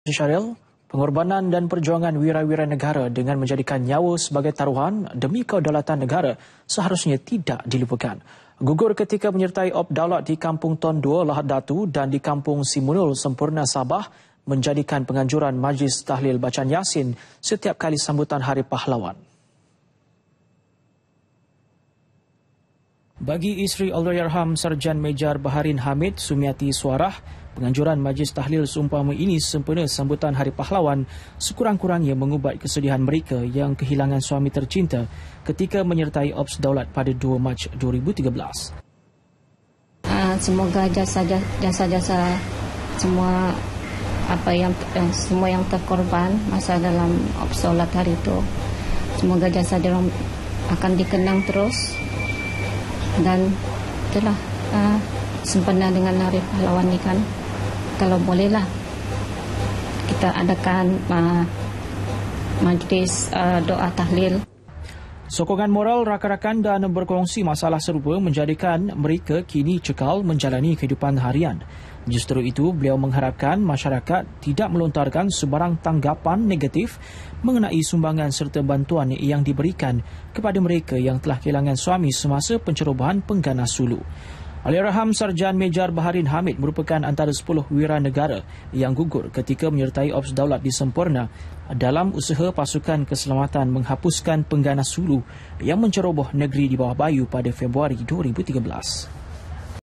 HARGAI PERJUANGAN WIRA NEGARA [31 JULAI 2015]
Pengorbanan dan perjuangan wira-wira negara dengan menjadikan nyawa sebagai taruhan demi kedaulatan negara, seharusnya tidak dilupakan.

Gugur ketika menyertai Op Daulat di Kampung Tanduo, Lahad Datu dan di Kampung Simunul, Semporna, Sabah, menjadikan penganjuran majlis tahlil bacaan yasin, setiap kali sambutan Hari Pahlawan.